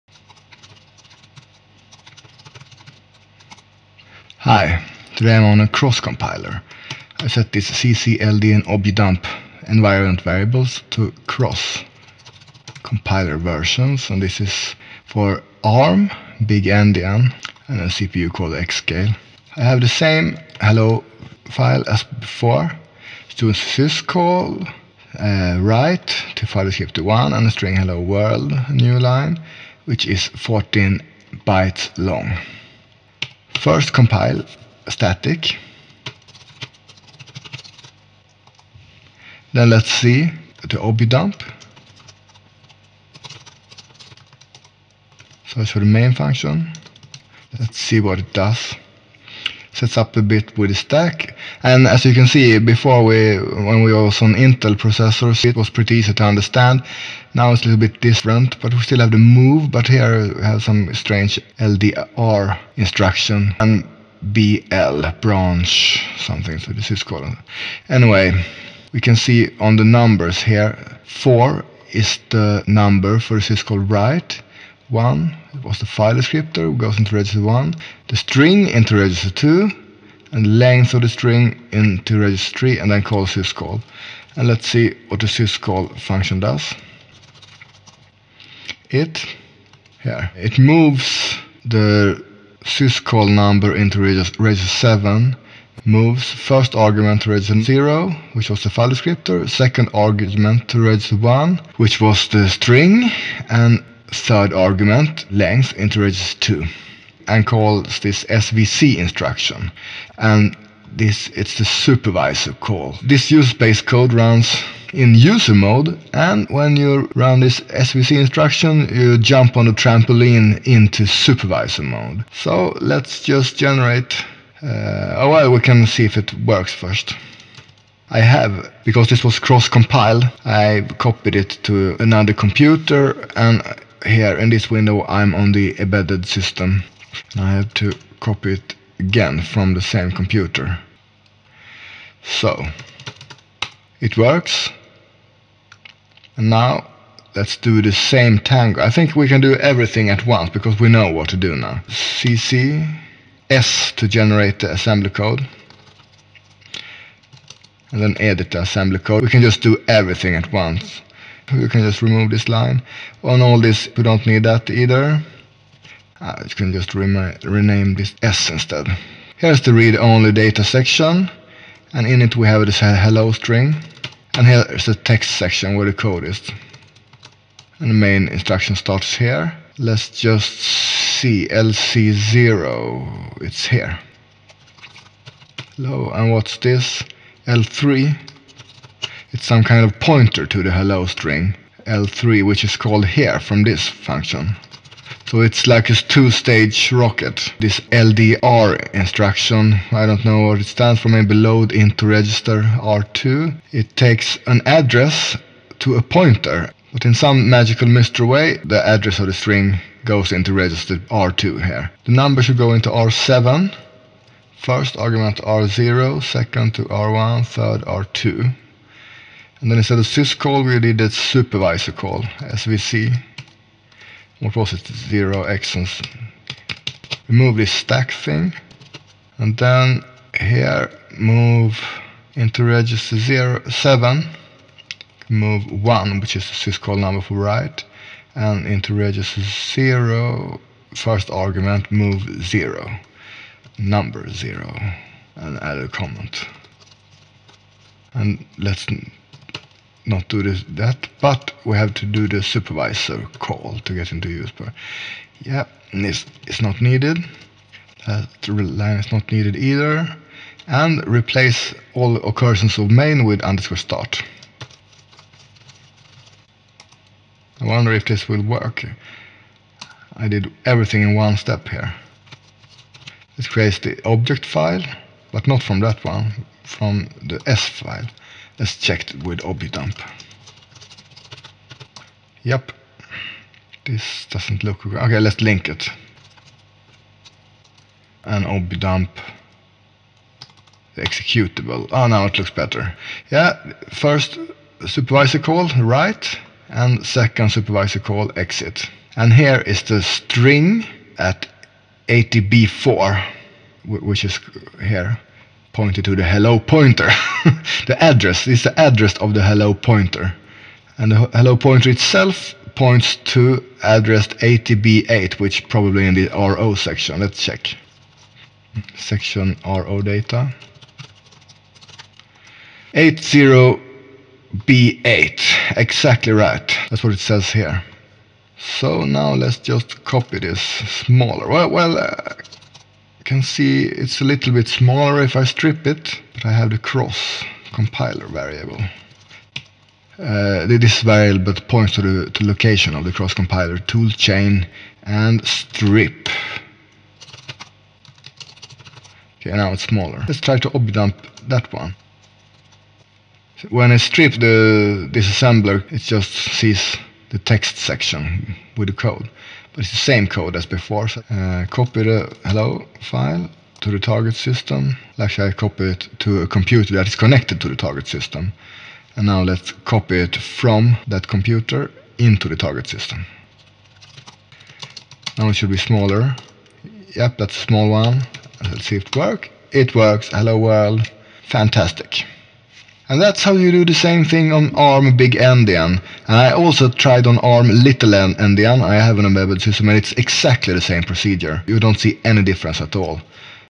Syscalls Part 4. Linux (ARM eabi)
Part 4 of a homemade screencast series on syscalls.

This time the same Hello World program is disassembled and translated to syscalls for an XScale ARMv5 processor (big endian) using EABI.

On ARM the syscall jump is done using the svc instruction (previously swi).